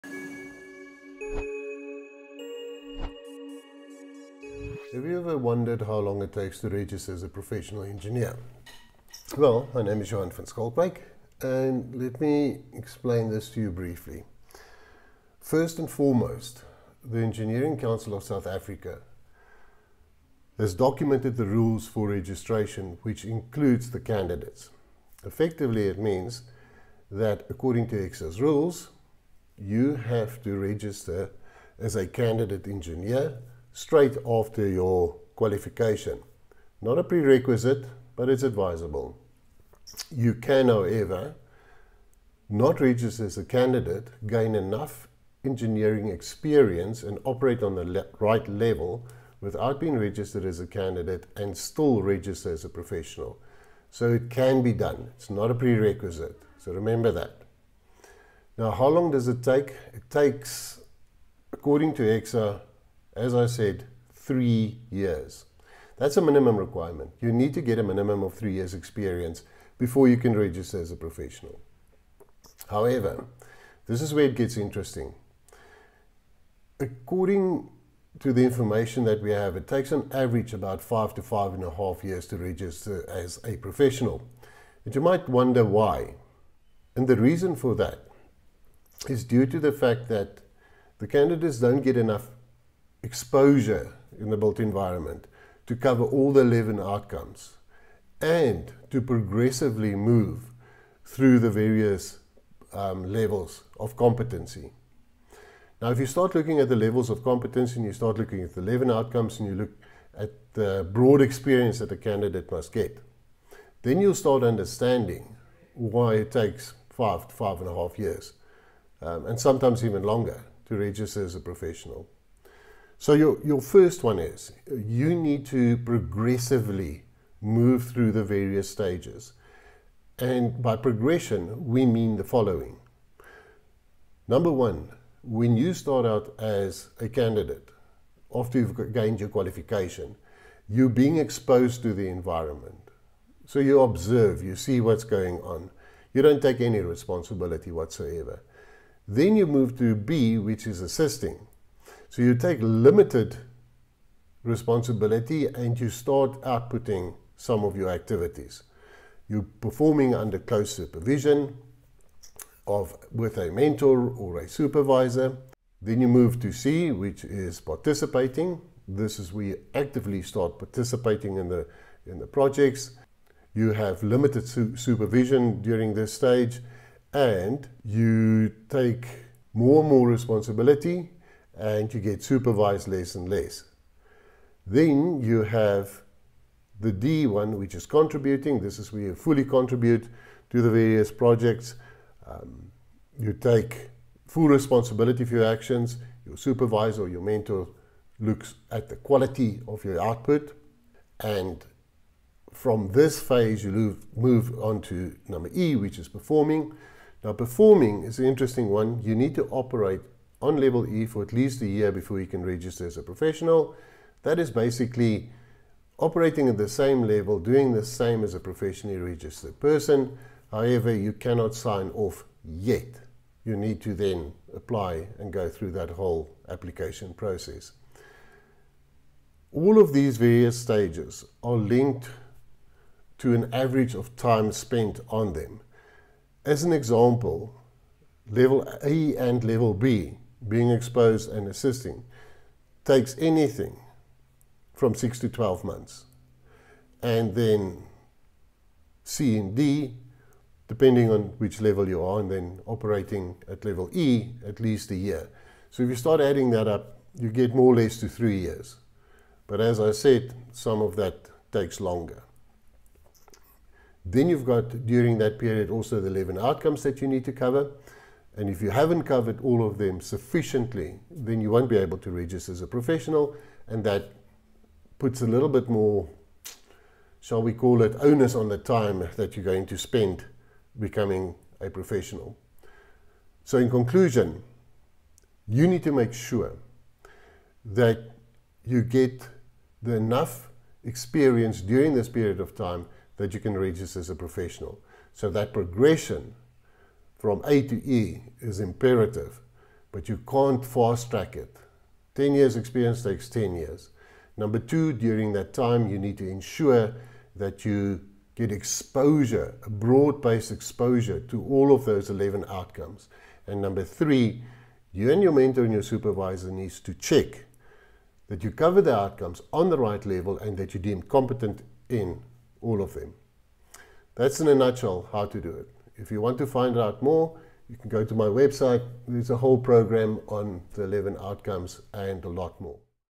How long does it take to become a professional engineer?
In this video we find out how long it takes to become a professional engineer, you might be surprised.